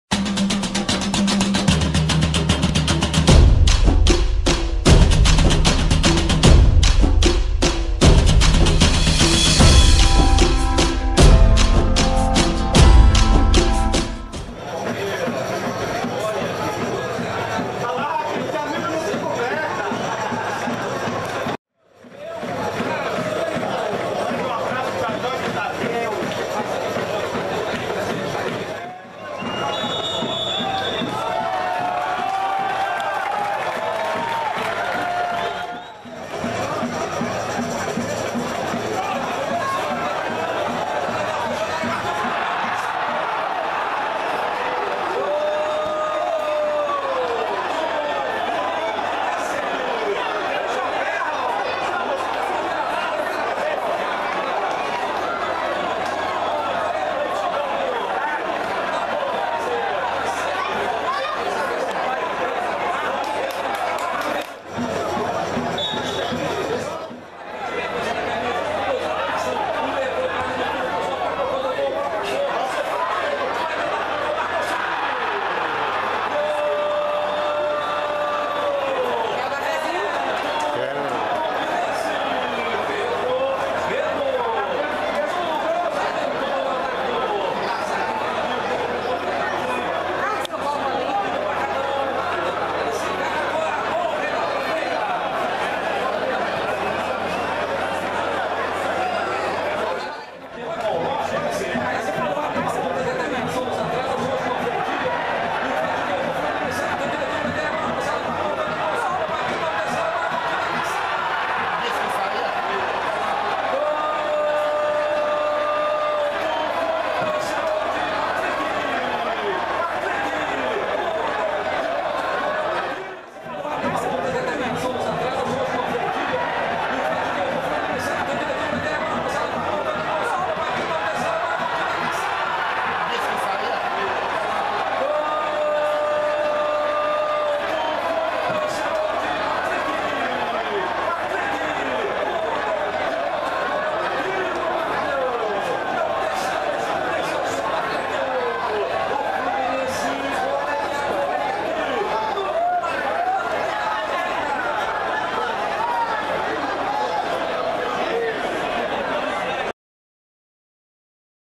Vitória 0 X 3 Fluminense (RJ). Brasileiro Sub 20. Gols de Daniel, Pedro e Patrick. (2/9/2015)
Fluminense vence Vitória e é campeão brasileiro sub-20. Fotos: Gabriela Simões
O Fluminense venceu o Vitória, dentro do Barradão, na tarde desta quarta-feira (2), por 3 a 0, e conquistou o título do Campeonato Brasileiro Sub-20. Os gols da partida foram marcados por Daniel, Pedro e Patrick. O tricolor carioca terminou a competição invicto, com dez vitórias e dois empates. Fotos: Gabriela Simões